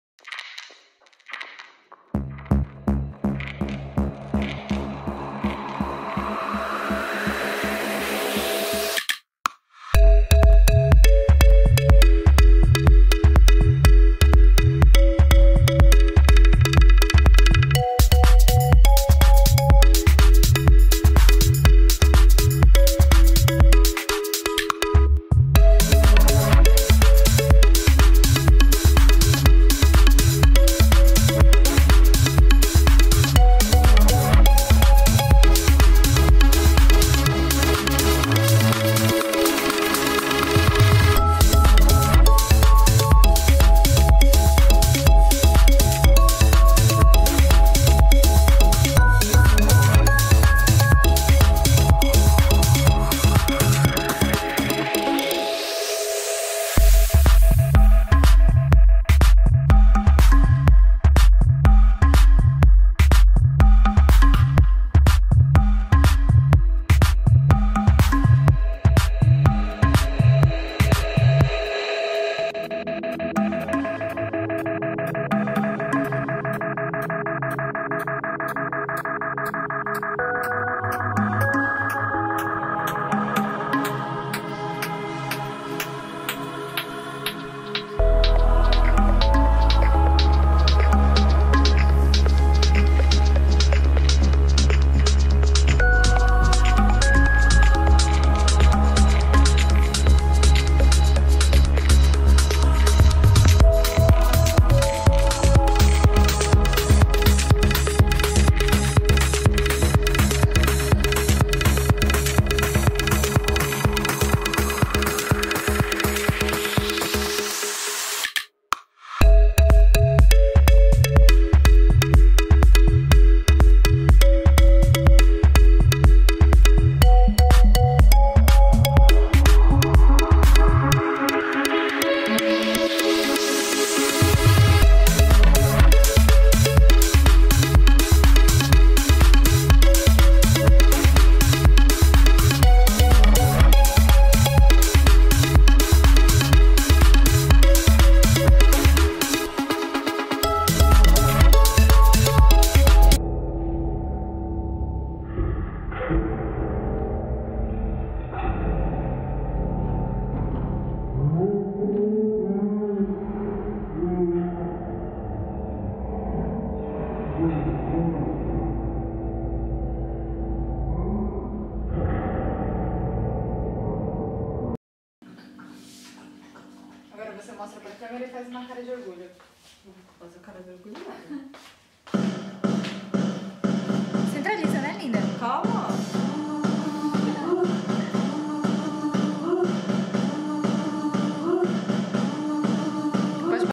Timelapse "Pintando a Noite" em tela
Posca, Sharpie, Guache, tela, pincel, lápis de cor e verniz em tela.